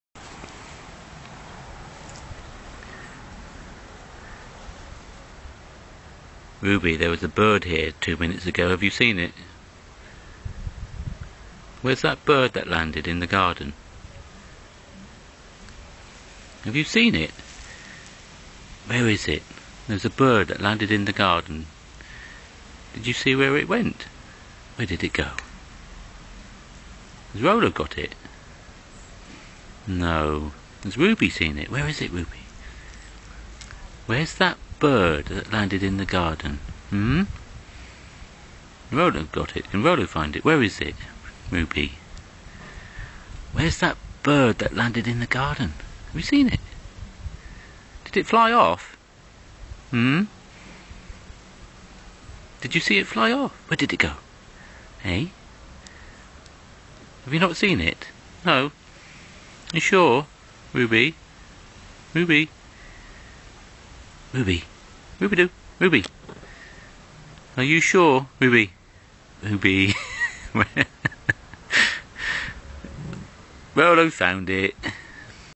Ruby where's that bird
Found my pet dog with bird feather stuck to her mouth and had to film it. tried everything to turn video 90degs but no joy!